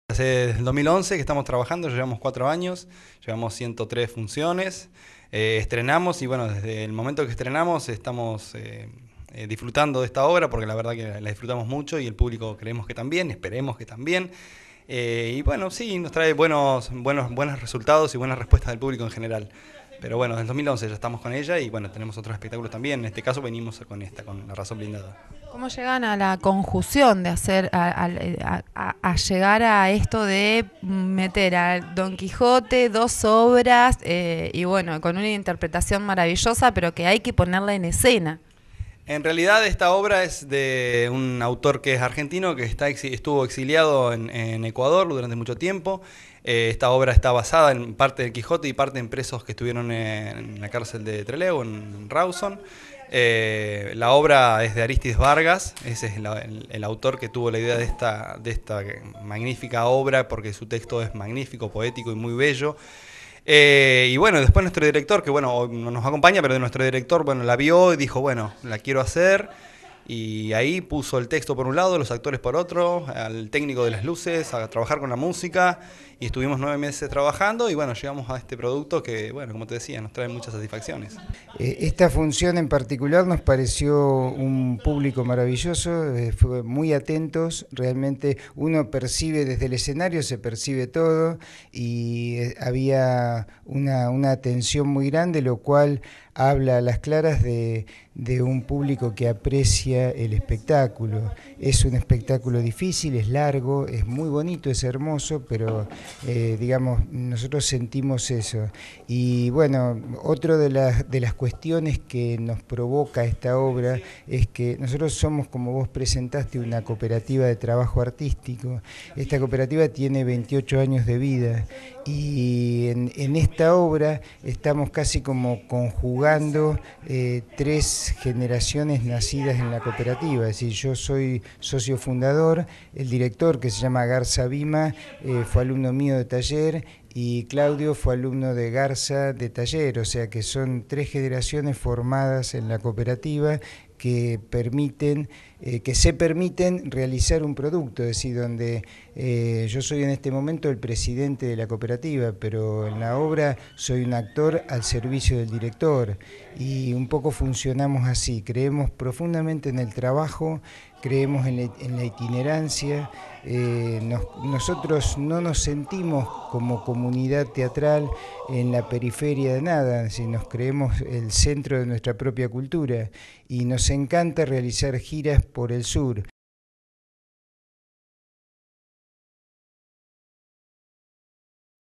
LA RAZÓN BLINDADA
SE PRESENTÓ EN LA CASA DE LA CULTURA “LA RAZÓN BLINDADA”, DE  ARÍSTIDES VARGAS. BASADA EN  “QUIJOTE DE CERVANTES” Y “ LA VERDADERA HISTORIA DE SANCHO PANZA” DE KAFKA.
CLAUDIO GRANADOS
ACTOR

SE PRESENTÓ EN LA CASA DE LA CULTURA

LA HORMIGA CIRCULAR
COOPERATIVA DE TRABAJO

CARLOS MAZZOLO
ACTOR